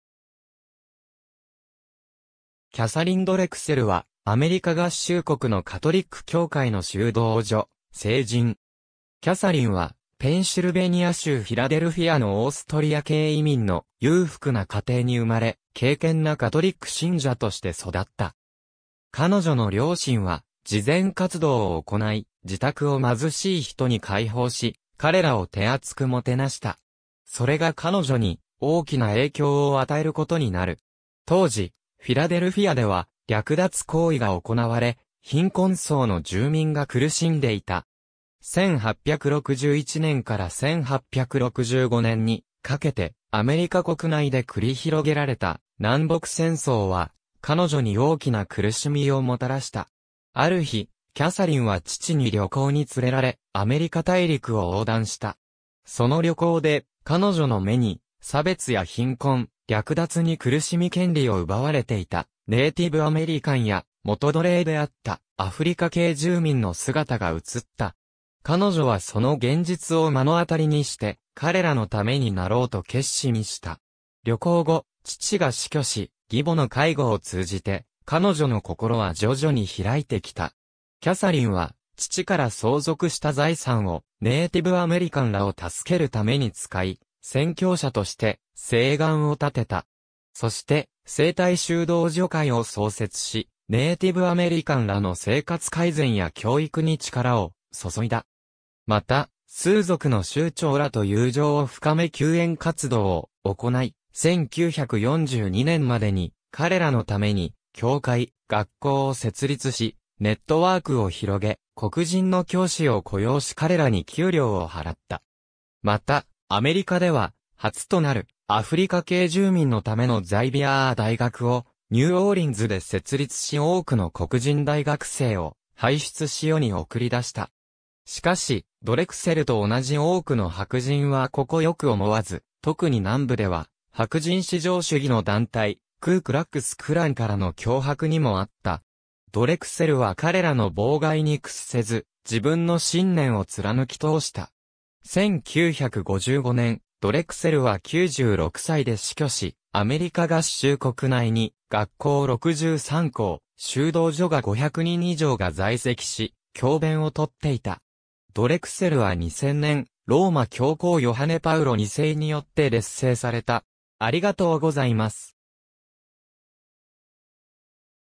キャサリン・ドレクセル
キャサリン・ドレクセル（Katharine Drexel、1858年11月26日 - 1955年3月3日）は、アメリカ合衆国のカトリック教会の修道女、聖人。
キャサリンはペンシルベニア州フィラデルフィアのオーストリア系移民の裕福な家庭に生まれ、敬虔なカトリック信者として育った。
彼女の両親は慈善活動を行い、自宅を貧しい人に開放し、彼らを手厚くもてなした。
それが彼女に大きな影響を与えることになる。
当時、フィラデルフィアでは略奪行為が行われ、貧困層の住民が苦しんでいた。
1861年から1865年にかけてアメリカ国内で繰り広げられた南北戦争は、彼女に大きな苦しみをもたらした。
ある日、キャサリンは父に旅行に連れられ、アメリカ大陸を横断した。
その旅行で彼女の目に差別や貧困、略奪に苦しみ権利を奪われていたネイティブ・アメリカン（先住アメリカ人、インディアン）や元奴隷であったアフリカ系住民（黒人）の姿が映った。
彼女はその現実を目の当たりにして、彼らのためになろうと決心した。
旅行後、父が死去し、義母の介護を通じて彼女の心は徐々に開いてきた。
キャサリンは父から相続した財産をネイティブ・アメリカンらを助けるために使い、宣教者として誓願を立てた。
そして「聖体修道女会」を創設し、ネイティブ・アメリカンらの生活改善や教育に力を注いだ。
また、スー族の酋長らと友情を深め救援活動を行い、1942年までに彼らのために教会、学校を設立しネットワークを広げ、黒人の教師を雇用し彼らに給料を払った。
また、アメリカでは初となるアフリカ系住民のためのザイヴィアー大学をニューオーリンズで設立し多くの黒人大学生を輩出し世に送り出した。
しかし、ドレクセルと同じ多くの白人（アングロサクソン、アイルランド系らをさす）はここよく思わず、特に南部では白人至上主義の団体、クー・クラックス・クラン（KKK）からの脅迫にもあった。
ドレクセルは彼らの妨害に屈せず、自分の信念を貫き通した。
1955年、ドレクセルは96歳で死去し、アメリカ合衆国内に学校63校、修道女が500人以上が在籍し、教鞭をとっていた。
ドレクセルは2000年、ローマ教皇ヨハネ・パウロ2世によって列聖された。